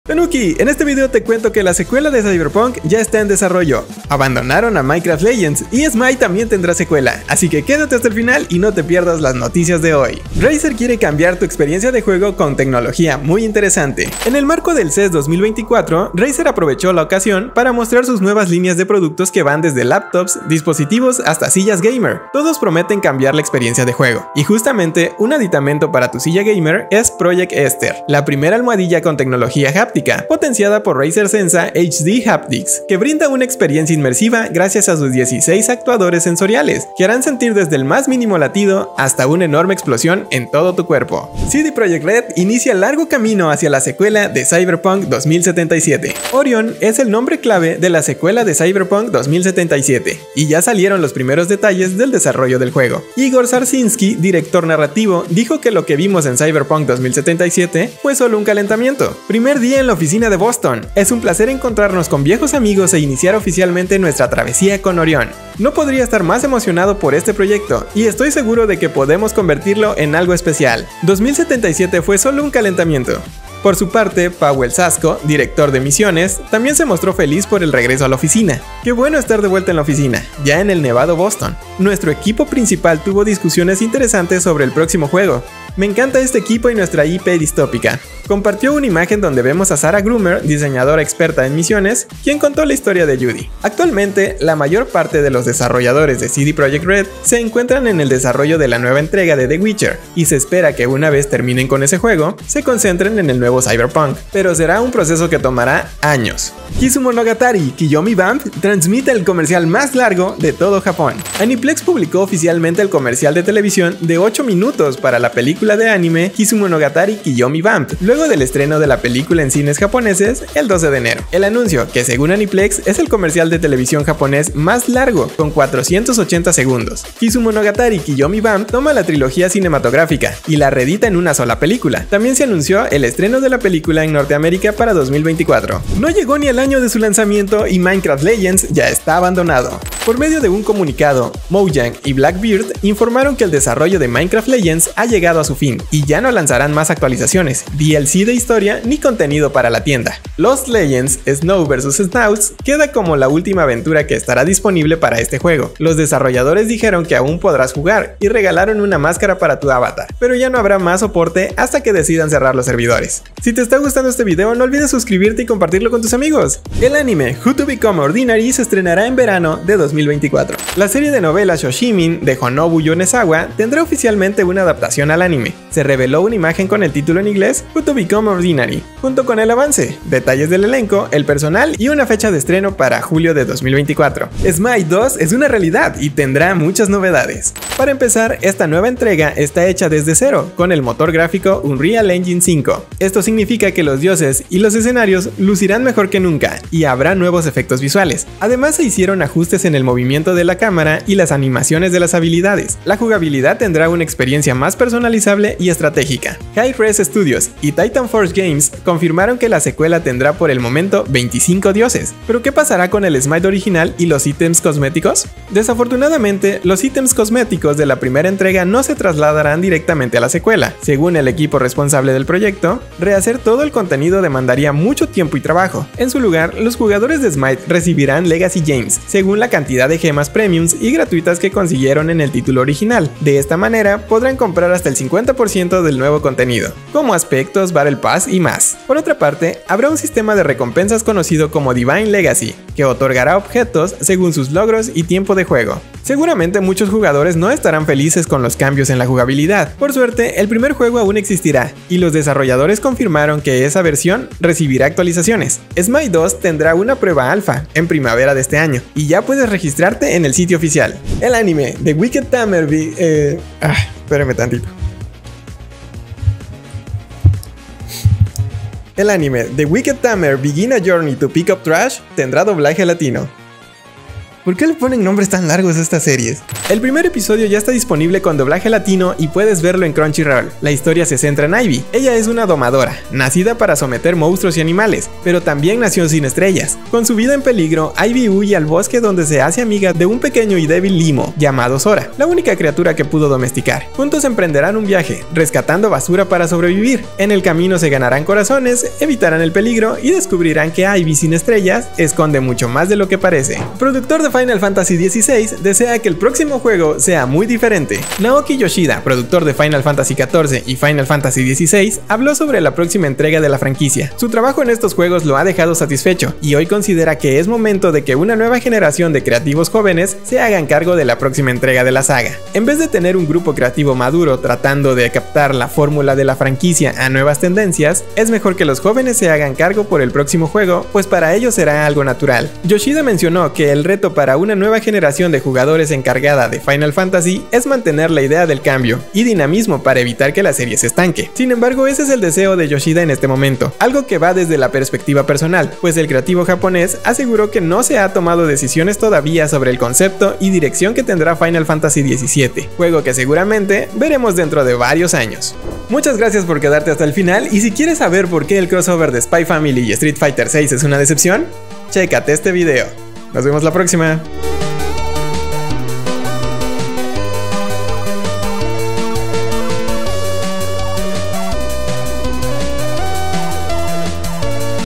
¡Cyberpunk 2 "Orion" ya está en desarrollo! Esto es que lo sabemos.
Tanuki en este vídeo te cuento que la secuela de Cyberpunk 2077 ya está en desarrollo, abandonaron a Minecraft Legends, SMITE también tiene secuela, así  Quédate hasta el final y no te pierdas las noticias de hoy.

1. *Razer: Revoluciona tu Juego con PROJECT ESTHER 🔥*
Razer se roba el show en el CES2024 con productos que cambiarán tu forma de jugar. Destacamos PROJECTESTHER, la almohadilla con tecnología háptica que promete una experiencia de juego inigualable. ¡Descubre cómo llevar tu inmersión al siguiente nivel!

2. *Cyberpunk 2077: ¡Secuela “Orion” en Desarrollo! 🌐*
CD Projekt Red comparte emocionantes detalles sobre “Orion”, la secuela de Cyberpunk2077. El equipo promete superar lo visto en el juego original. ¿Qué expectativas tienes para este nuevo capítulo en Night City? ¡Déjanos saber en los comentarios!

3. *Kizumonogatari: ¡Comercial de 8 Minutos! 🎬*
Aniplex sorprende con el comercial más largo de Japón para KizumonogatariKoyomiVamp. Descubre detalles sobre la reedición cinematográfica y la llegada de la película a Norteamérica en 2024. ¿Eres fan del anime? ¡Este avance es para ti!

4. *Fin de Minecraft Legends: Última Aventura Anunciada 🕹️*
Mojang y Blackbird dejan de desarrollar MinecraftLegends. Conoce los detalles sobre el cierre y la última aventura disponible. ¿Qué opinas de esta decisión? Comparte tu perspectiva en los comentarios.

5. *How to Become Ordinary: Nuevo Anime en Verano 2024 ☀️*
La adaptación de la serie de novelas "Shoshimin" llega con " HowtoBecomeOrdinary ". Descubre la trama de dos estudiantes que buscan una vida pacífica en la escuela secundaria, solo para enfrentarse a misteriosos incidentes. ¿Emocionado por este nuevo anime?

6. *SMITE 2: ¡La Secuela que lo Cambiará Todo! ⚡*
SMITE2 llega con cambios significativos en mecánicas y visual. Detalles sobre el motor UnrealEngine5, ajustes en la jugabilidad, y un sistema de recompensas único. ¿Qué opinas sobre los cambios? ¡Comparte tu perspectiva!

7. *The Weakest Tamer Began a Journey to Pick Up Trash con Doblaje Latino 🌍*
¡Anime con doblaje latino! Descubre la historia de Ivy y su viaje junto a un pequeño limo llamado Sora en el anime: TheWeakestTamerBeganaJourneytoPickUpTrash . ¿Cómo se ganarán el corazón de la audiencia mientras recogen basura para sobrevivir? ¡Entérate de todos los detalles!

8. *Final Fantasy XVII: Cambios y Nueva Generación 🌌*
NaokiYoshida, productor de FinalFantasy, comparte su deseo de que la próxima entrega FinalFantasyXVII sea liderada por una nueva generación de creativos. ¿Cómo te gustaría que evolucionara la franquicia? ¡Comparte tus ideas en los comentarios!